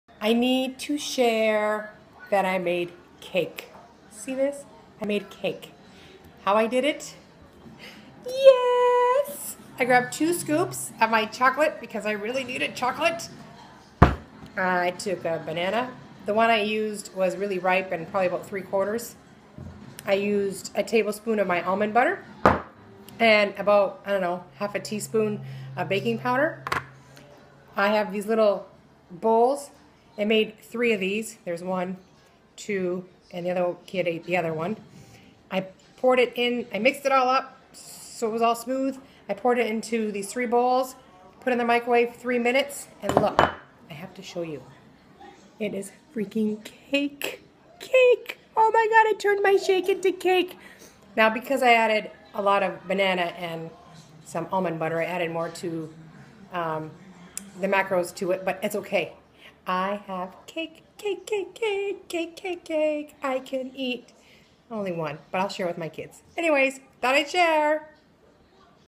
Protein Chocolate Cake
Sometimes you just need a piece of cake!!

2 scoops Isalean
1 ripe banana
1 tbsp almond butter
1/2 tsp of baking power

Pour in 3 mugs and microwave 3 minutes